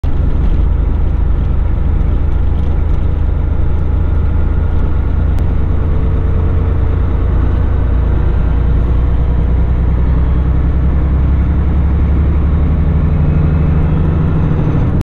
She's sinking Cap'n
On my way to Golden Meadow, Louisiana and I came upon what looks like a boat crash. One was sinking.